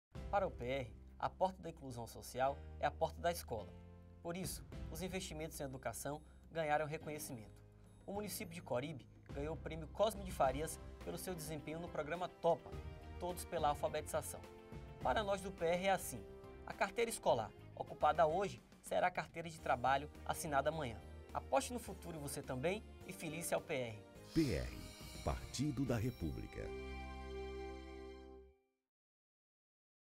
Prefeito de Coribe Manuel Rocha
Fala do Prefeito de Coribe-Bahia, Manuel Rocha do PR